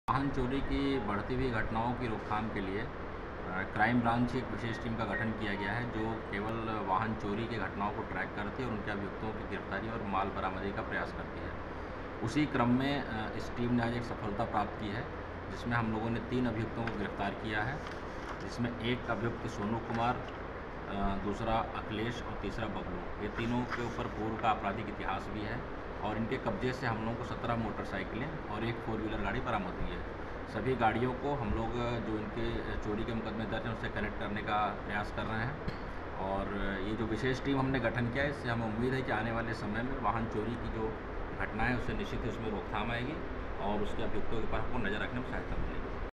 फिरोजाबाद : अंतर्राज्यीय वाहन चोर गैंग के तीन बदमाश गिरफ्तार, 17 बाइक और कार बरामद
फिरोजाबाद :  सिरसागंज पुलिस ने वाहन चोरी करने वाले एक अंतर्राज्यीय गिरोह का खुलासा किया. चोरी करने वाले इस गैंग के तीन आरोपियों को गिरफ्तार किया गया है. वहीं इनके पास ले 17 बाइक और कार भी बरामद की गई.